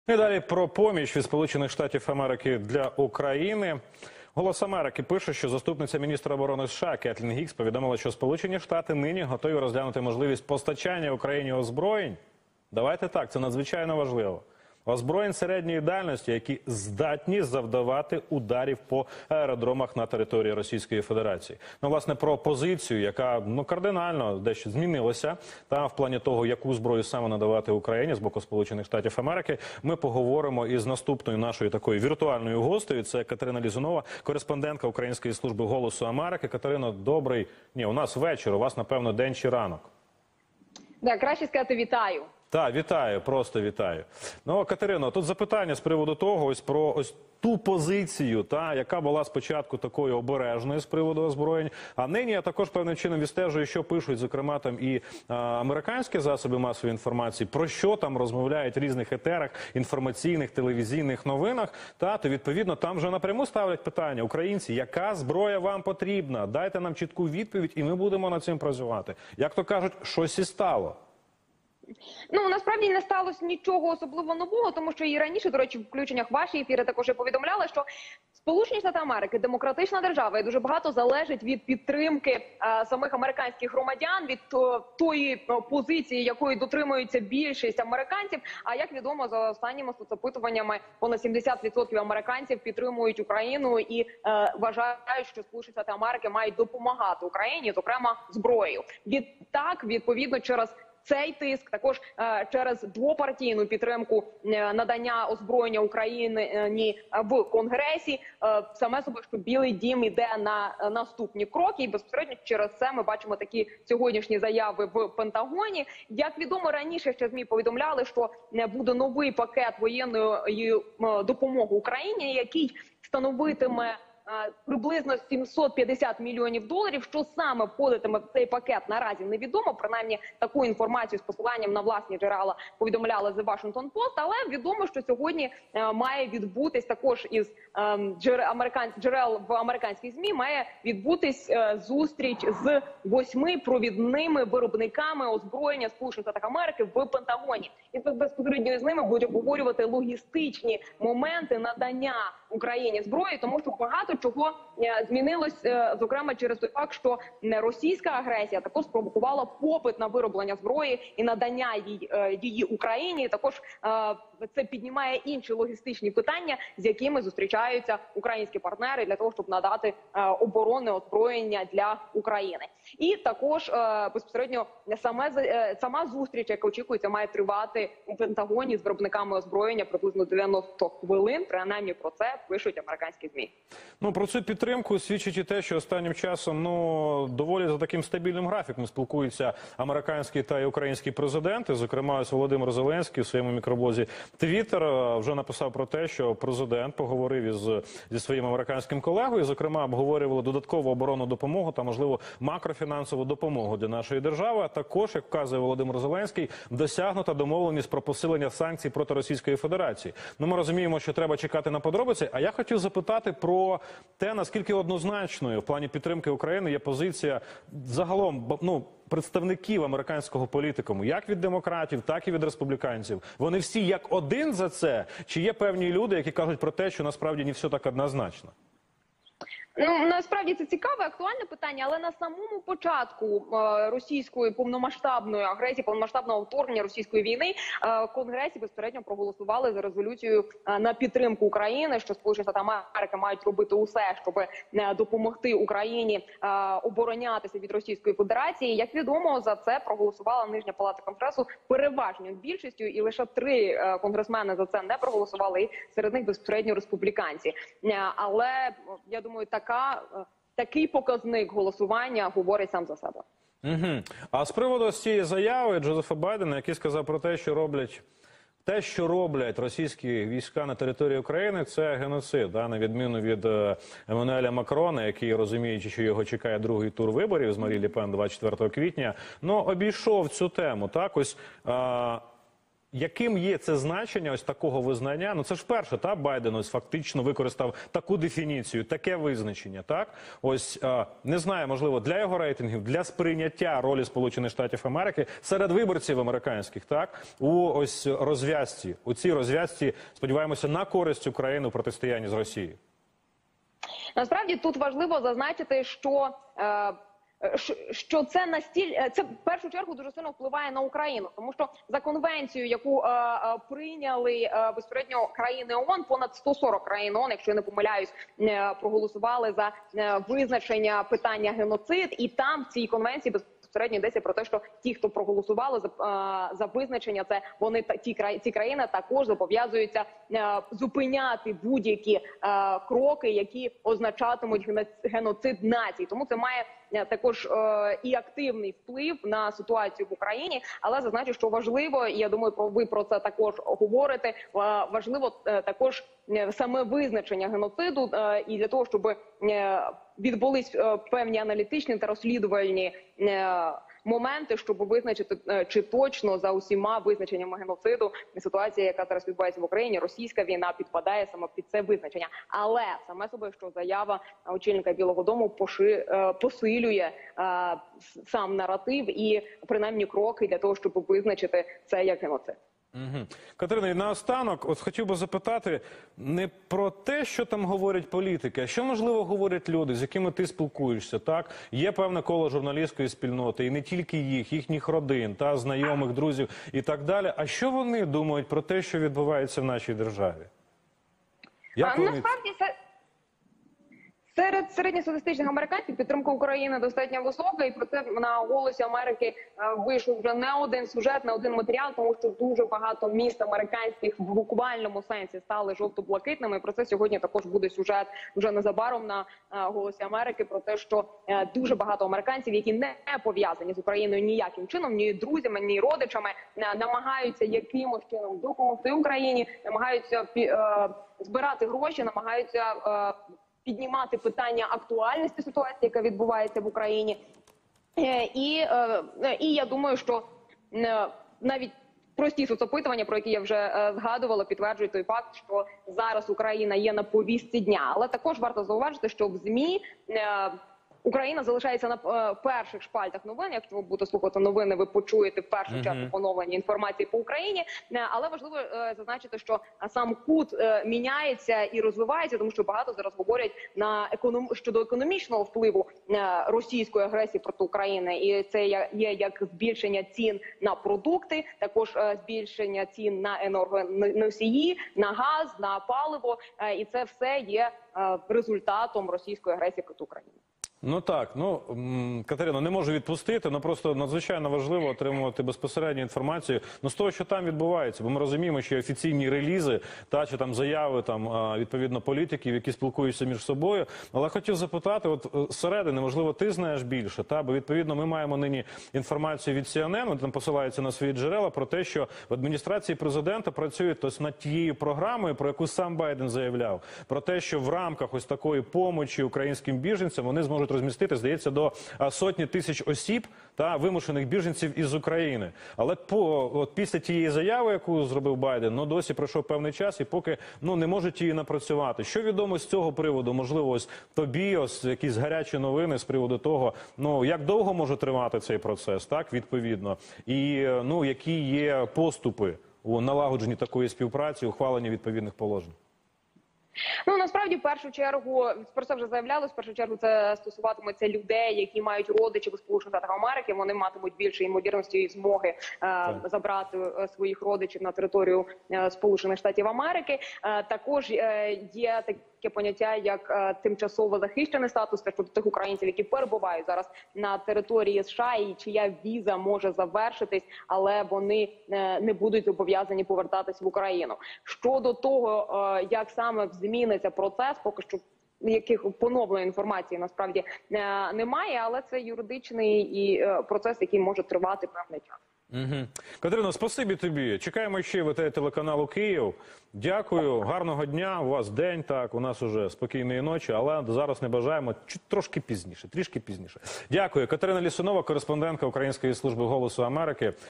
Зброї для України буде багато - США вирішують питання доставки
Катерина Лісунова, кореспондента української служби "Голос Америки" про черговий пакет американської допомоги від США.